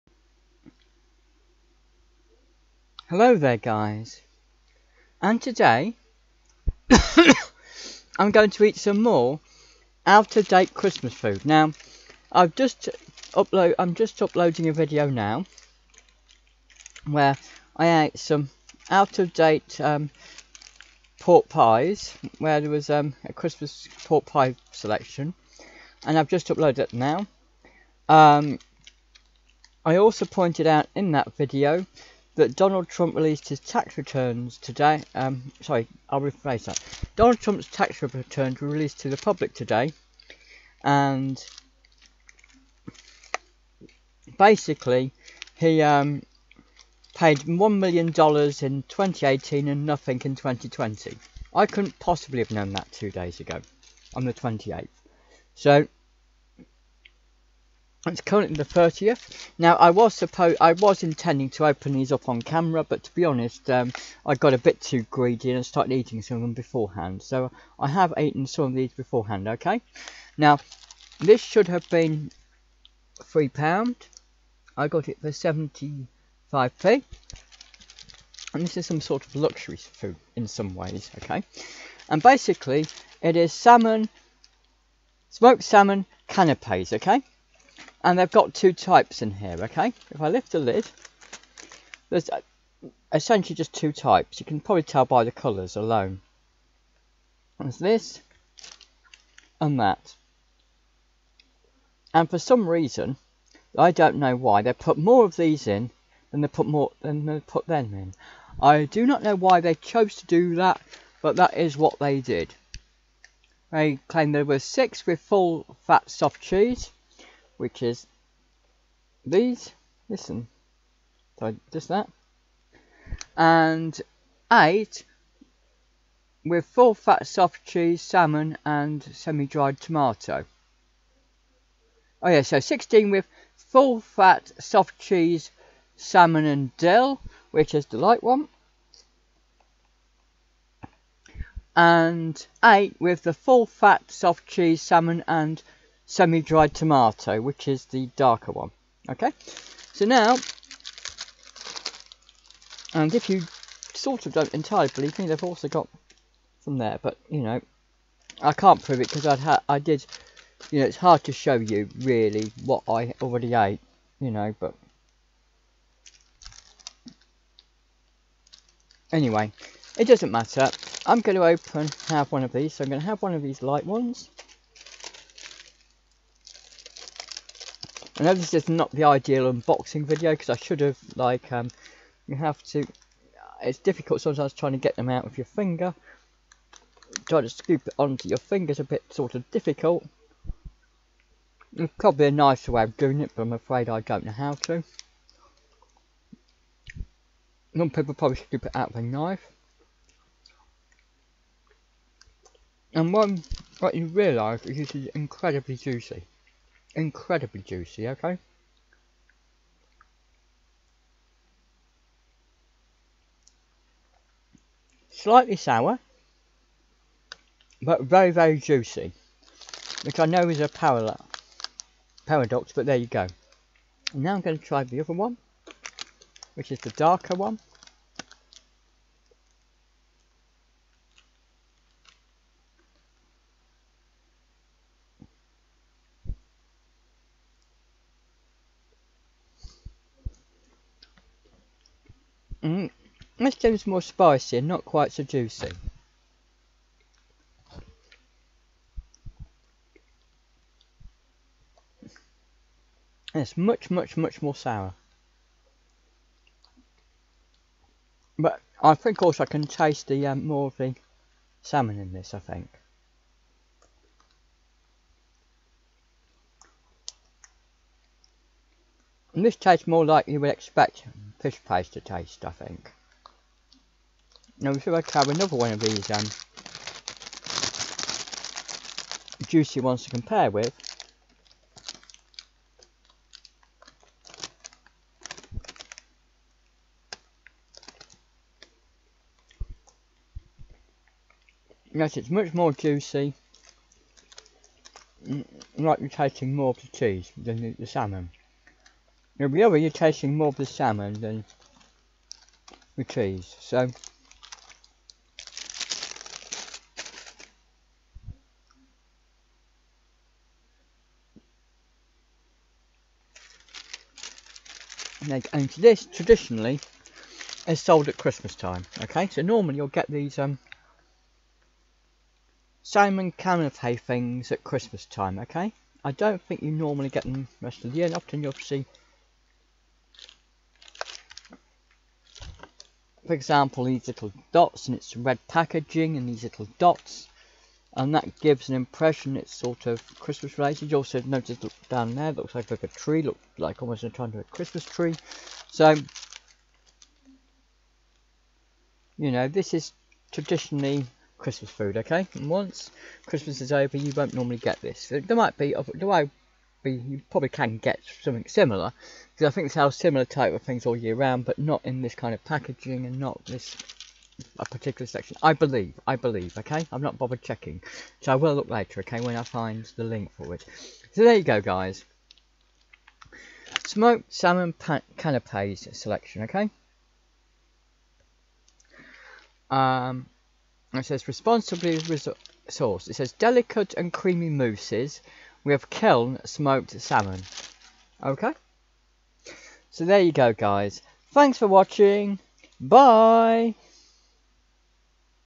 Santa Cliff Eats Out Of Date Christmas Food -Tesco Pack Of 24 Smoked Salmon Canapes
I eat some out of date Smoked Salmon Canapes from a special Christmas selection.

BBC News Article link about Donald Trump to prove I ate them out of date: